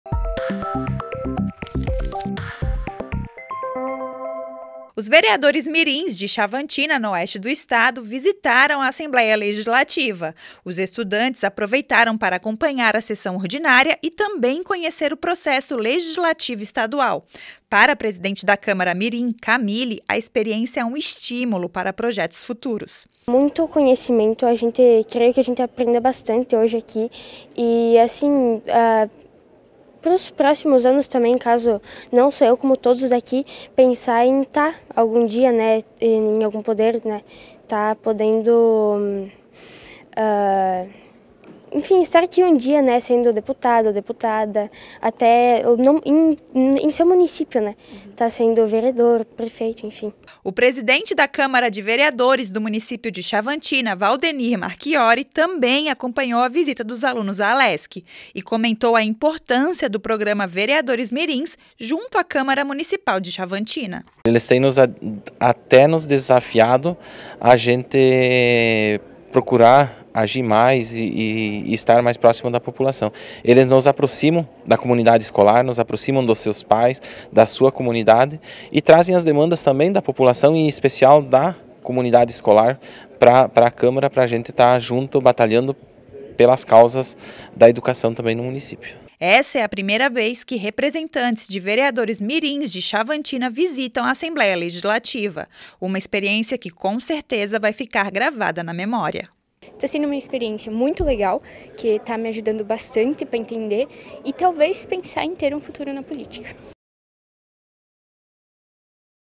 É Notícia - Vereadores Mirins de Xavantina visitam a Alesc - 13/09/22
Vereadores mirins de Xavantina, no oeste do estado, visitaram a Assembleia Legislativa. Os estudantes aproveitaram para acompanhar a sessão ordinária e conhecer o processo legislativo estadual. Para os jovens vereadores, estar no parlamento estadual, é uma experiência única e um incentivo para planos futuros.